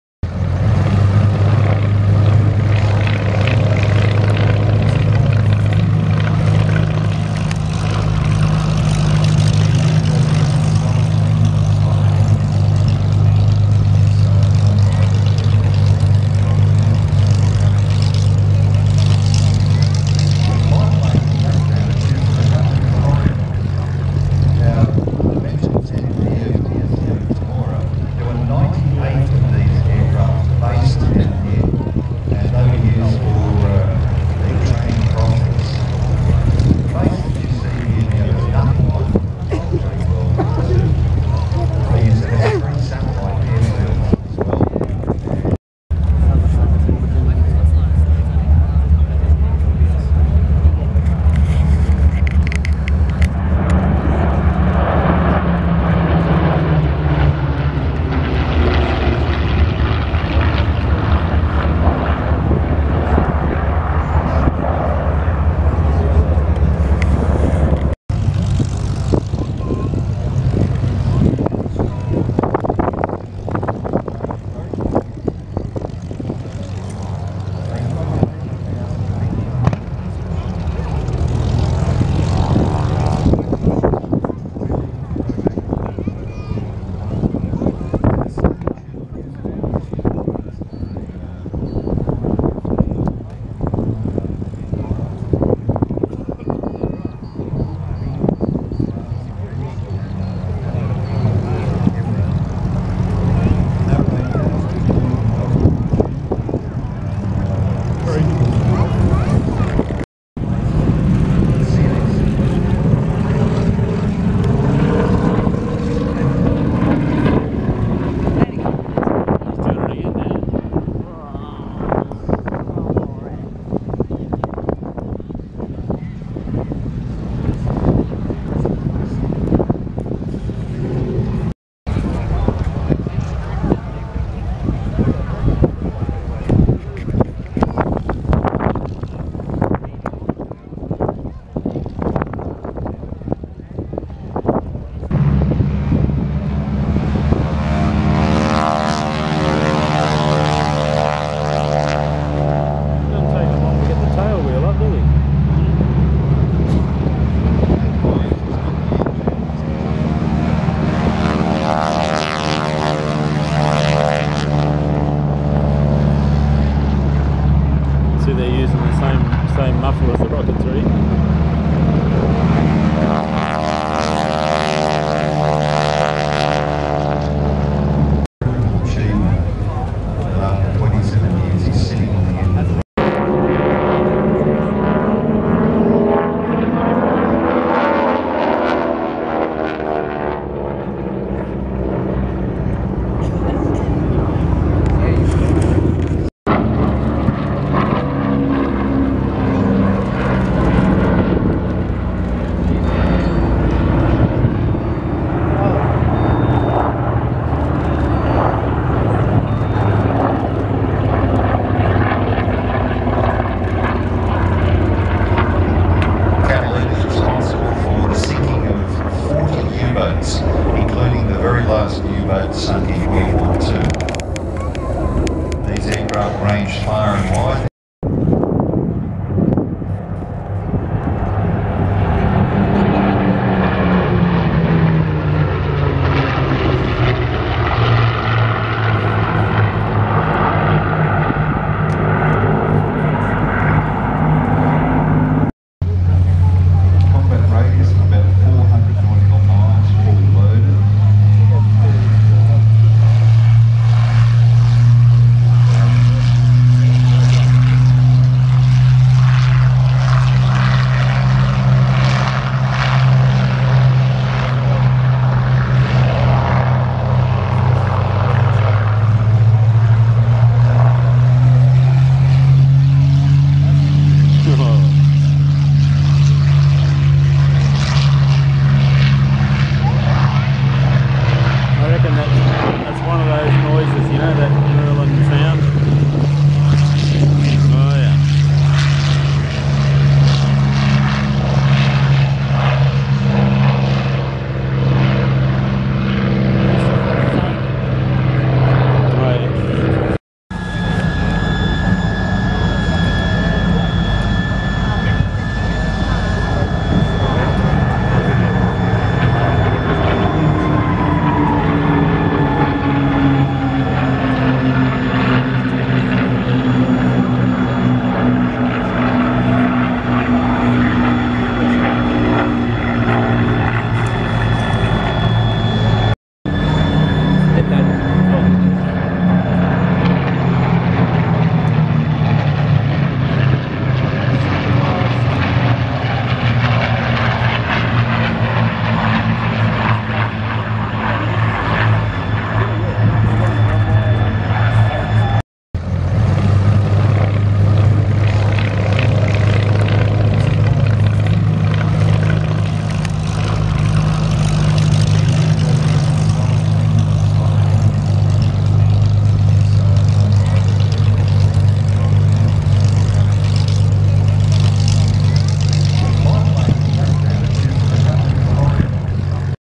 Warbirds Downunder Temora 2018
Went to the Warbirds Downunder Airshow in Temora last weekend. Took loads of pointless photos of specks in the sky and very dodgy videos of aircraft flying about. Loads f r=fun though and there  is nothing in the world like the sound of a V12 Merlin engine.